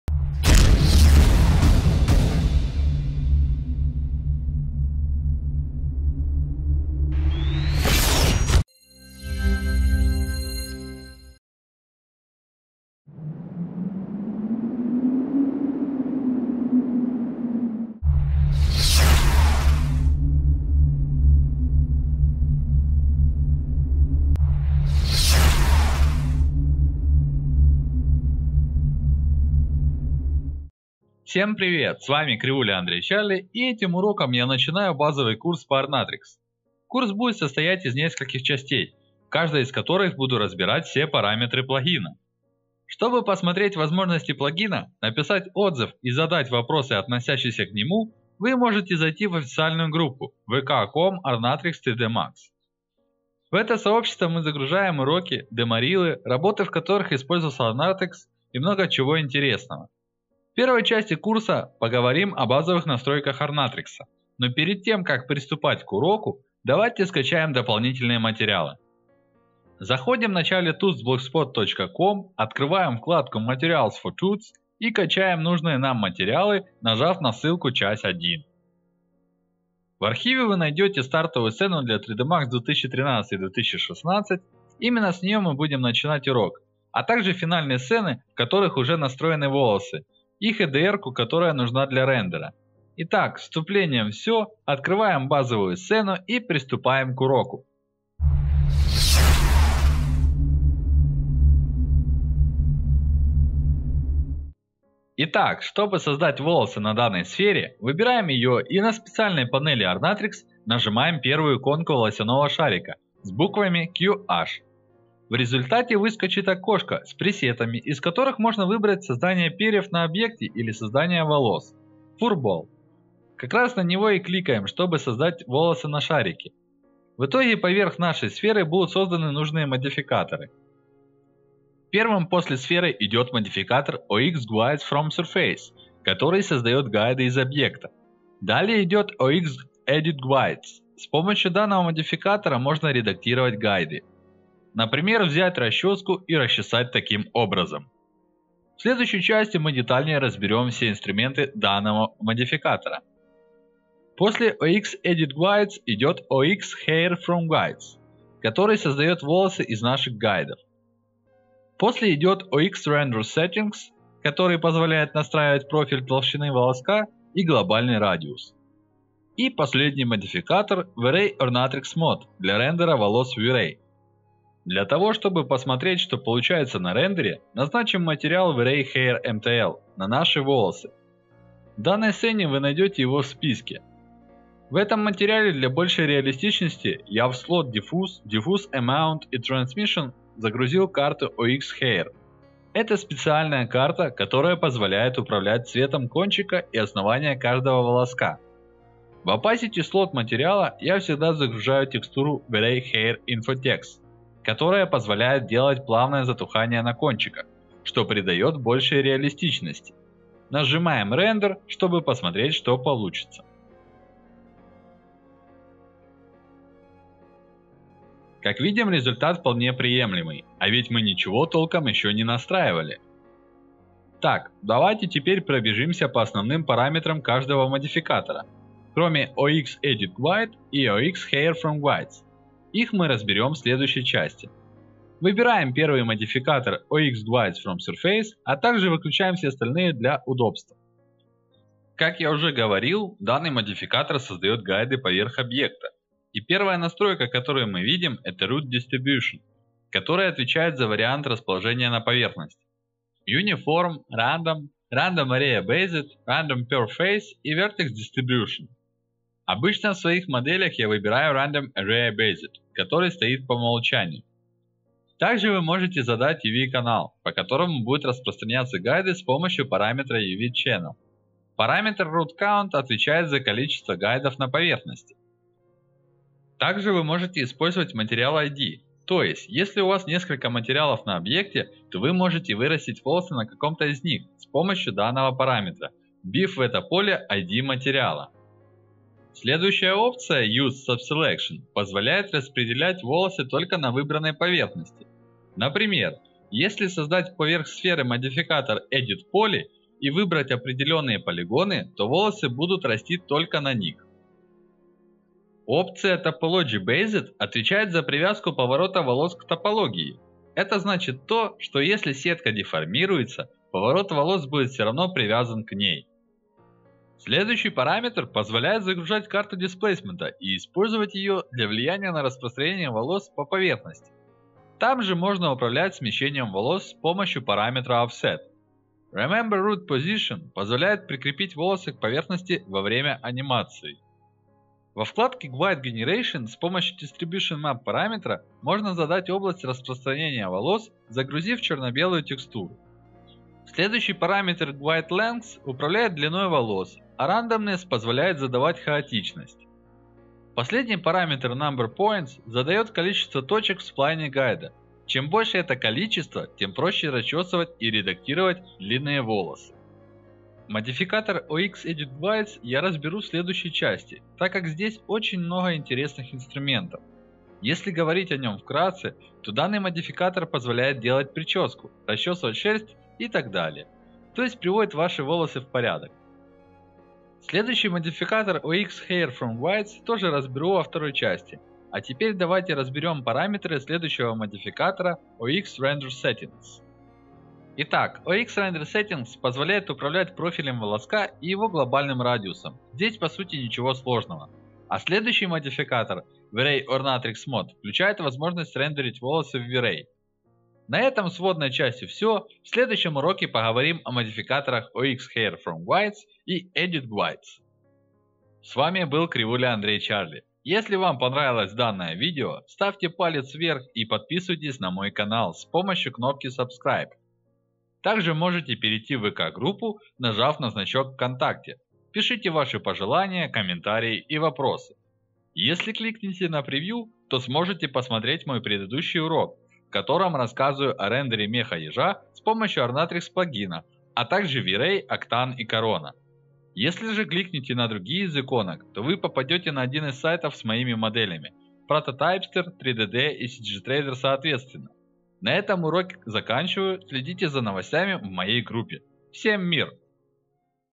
Ornatrix plugin. 3ds Max tutorial. Chapter 1.
A brief overview of standard modifiers and hair rendering in Vray using a preset.

Hello! I'm Krivulya Andrey aka Charly with you and today I present to you the first part of my basic course about the Ornatrix plugin, in which I show the settings of the main modifiers, as well as quick hair rendering using the preset and the OxTexture map.

     Video content:

00:00 - 00:27 - Intro

00:27 - 01:43 - Welcome and download additional materials

01:43 - 06:48 -  A brief overview of Ornatrix modifiers and hair rendering with the Vray preset

06:48 - 07:30 - The ending. Other tutorials preview.